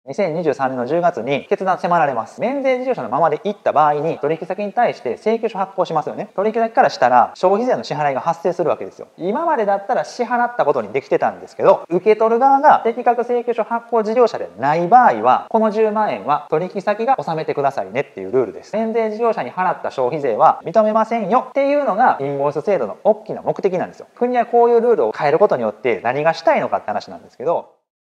インボイス制度が始まり免税事業者と取引すると損するのは課税事業者です！免税事業者との取引は要注意！
インボイス制度とその裏側！知らないと売上ダウンは確定です...